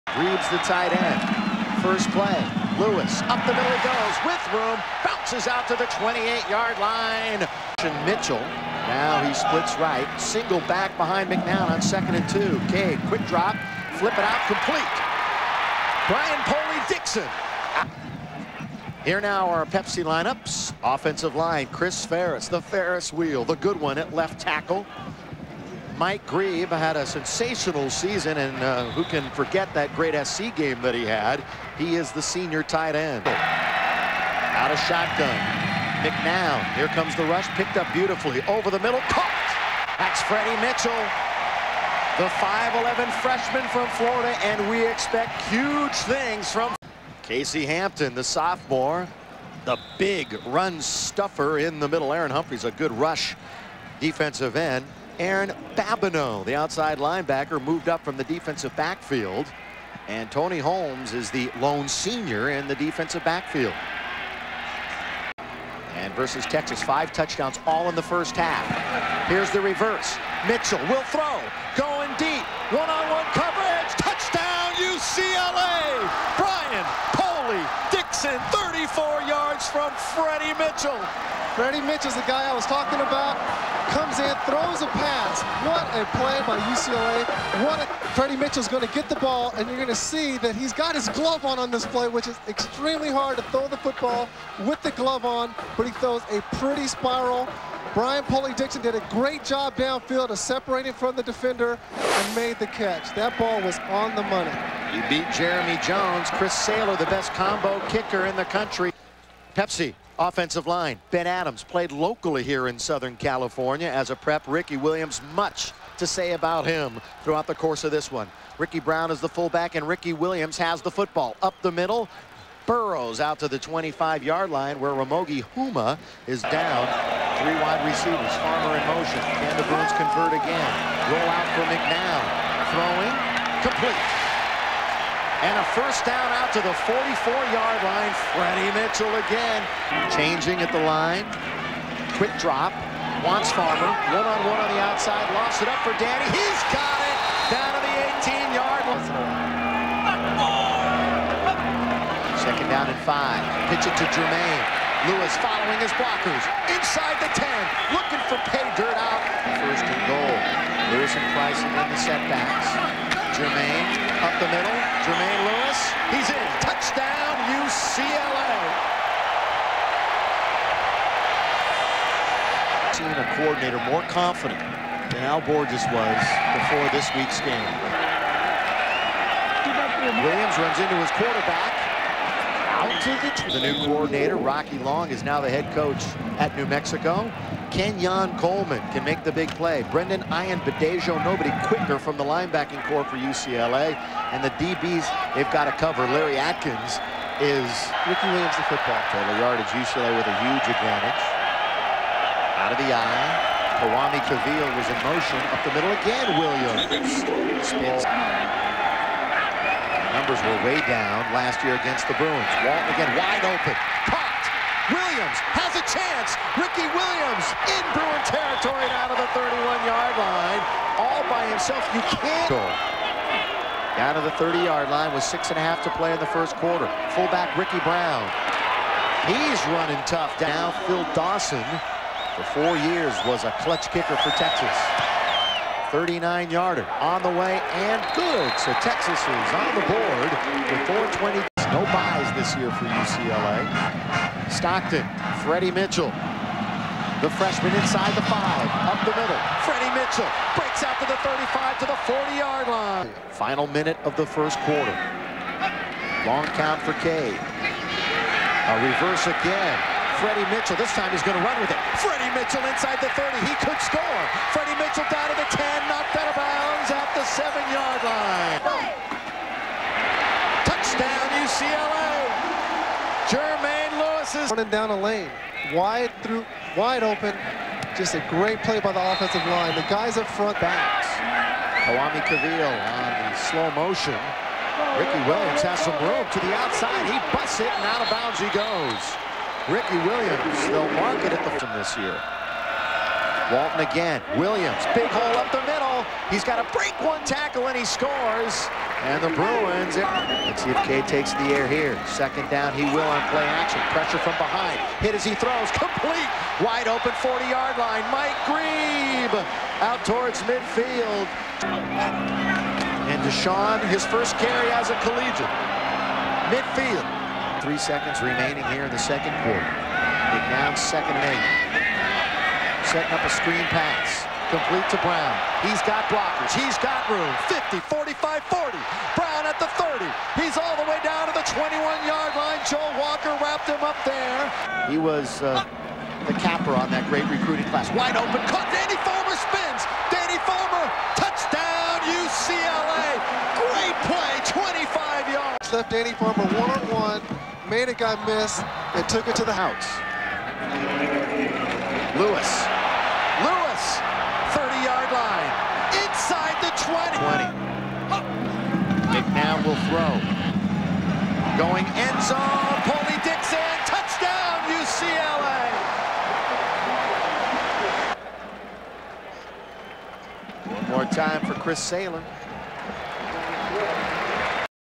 1998 Texas @ UCLA - First Half
UT arrived in Pasadena with revenge for Rout 66 on their minds, so much so that the blocking sleds had the #66 jersey, bringing a healthy stock of NFL talent -- Ricky Williams, Wane McGarity, Kwame Cavil, Sean Rogers, Casey Hampton, DD Lewis, Cedric Woodard, Aaron Humphrey and Quinten Jammer.  The result was again a disappointment, outmatched by a national title caliber UCLA team in full form in their first game.

First half

UCLA drives down the field on the opening drive which culminated in a reverse and WR pass by FR WR Freddie Mitchell to Brian Poli-Dixon. A highlight reel play executed with ease. UCLA 7-0

UCLA takes their next possession and with the help of a spectacular overhead catch by Dan Farmer -- thrown by Cade McNown - runs in for a 14-0 lead

UT counters with Ricky -- runs and an impressive screen pass. Ricky Brown with a nice run.The drive stalls and Stockton kicks a FG. UCLA 14-3

UCLA takes the KO return by Freddie Mitchell and another reverse with a run to set up another TD run. UCLA 21-3

Texas has a couple of solid Ricky runs but the drive stall.

UCLA takes the possession and with a long screen pass of their own sets up a TD pass where Farmer spins Quintin Jammer into an embarrassing pose for the easy TD. UCLA 28-3. Desaun Foster had his first college run included in the drive -- this talented FR RB had 59 TDs and 3300 yards rushing in his high school senior year, went on to play in the NFL.

Texas can barely execute anything outside of Ricky, so UCLA gets the ball and scores again 35-3

Combining the 1997 game (66-3) and this first half, UCLA outscores Texas 101-6